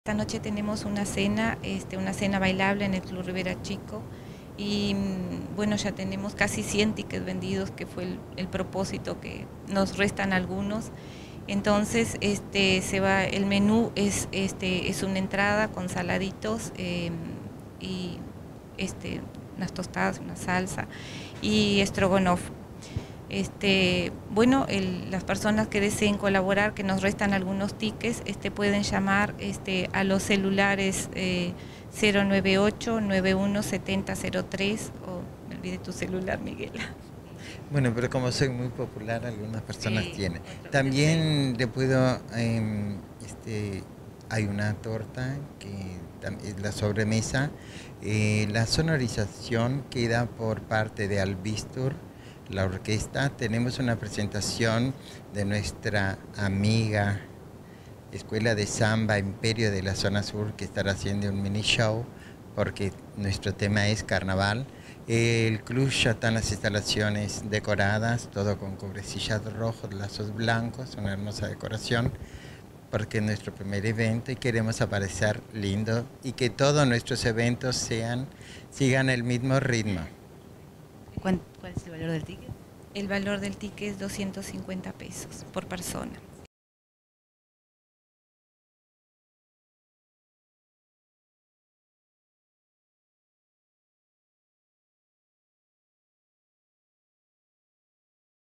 Escuela de samba fundada en el barrio Rivera Chico/17-07-2015
Con el fin de financiar la escuela “Nación Guerrera de Cuaró” se estará realizando una cena show en el club Rivera Chico hoy a partir de las 21 hs.